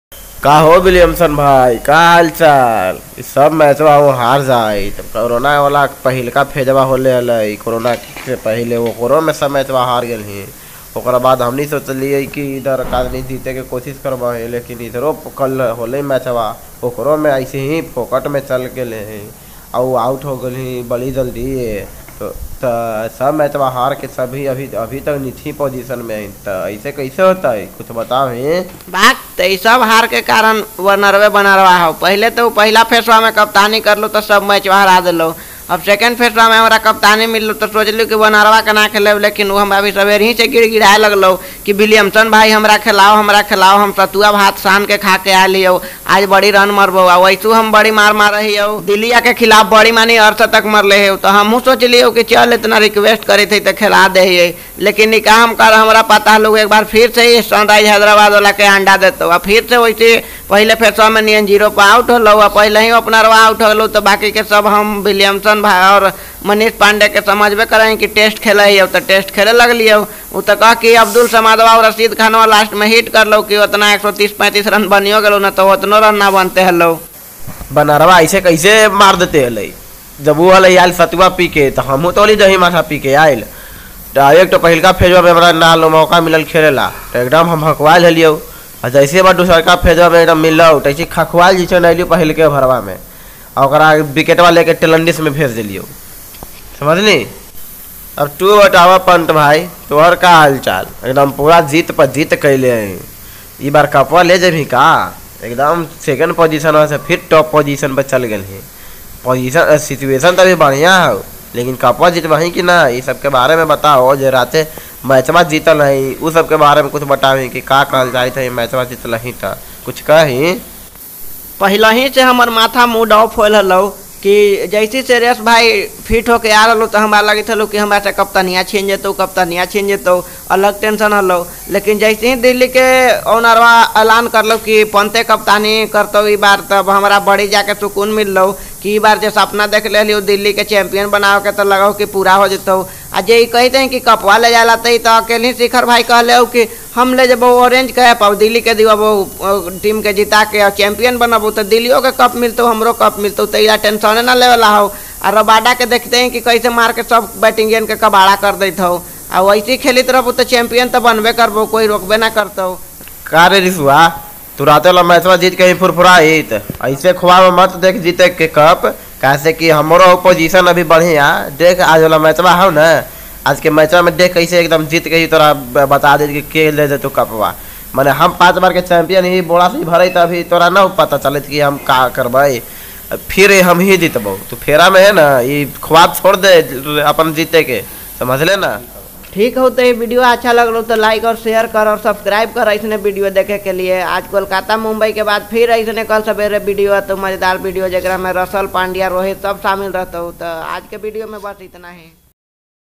Williamson Frustration After Losing Match ! मैच हारने के बाद हैदराबाद टीम की हुई दुर्दसा ! DC vs SRH
Dubbed By : - Satish Verma ,pawan Kumar
Edited By :- pawan kumar singh